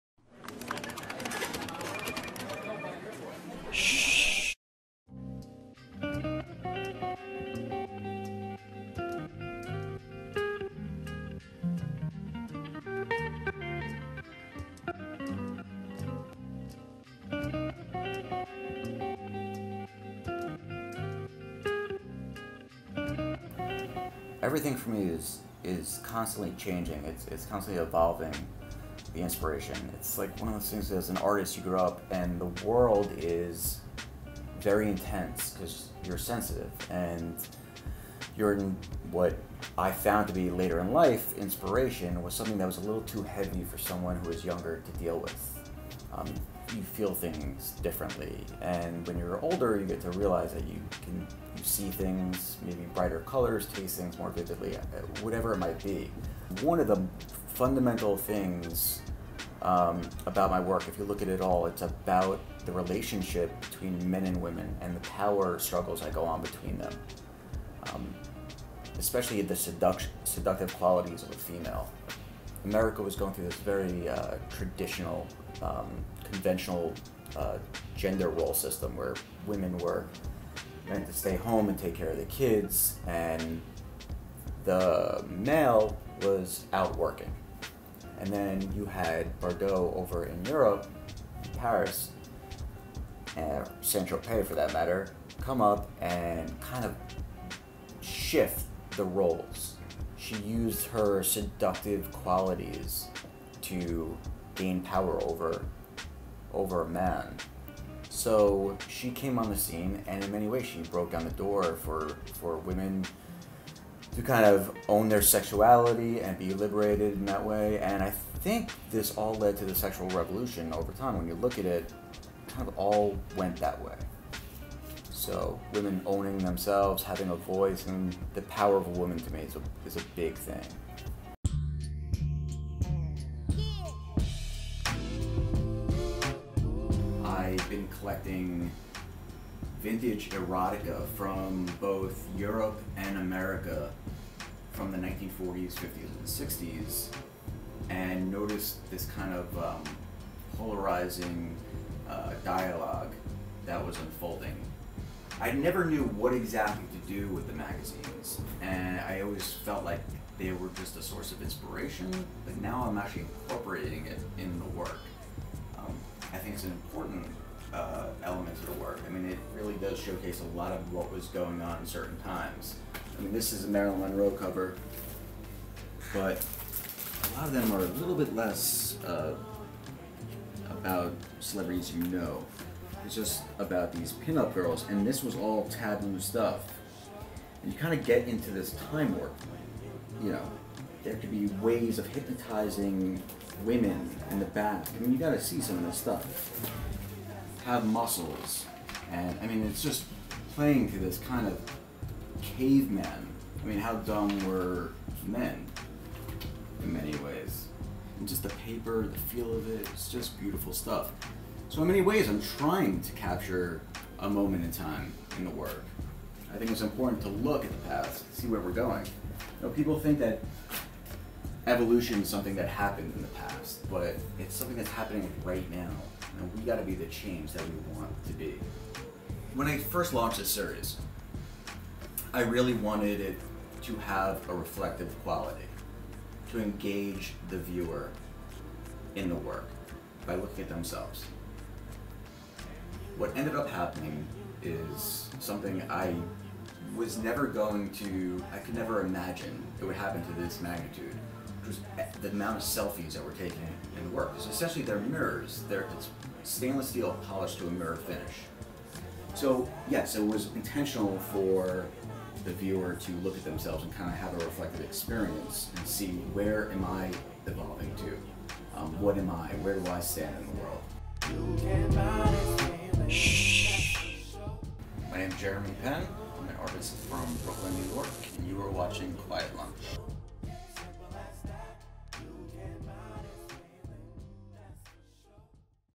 QUIET LUNCH MAGAZINE. | Jeremy Penn.
We recently visited Jeremy Penn at his studio in Brooklyn. His work is a visceral splash of color and iconography. Penn, with respects to the lasting trail of time, has his way with nostalgia—appropriating vintage magazine clipping, often bathing their imagery in a library hues. This formula is the result of Penn's understanding of evolution and the omniscient role it plays in our everyday lives.

Filming | Editing: Kareem Gonsalves | @Derkgphoto

Many thanks to: Jeremy Penn | @JeremyPenn

- QLM Media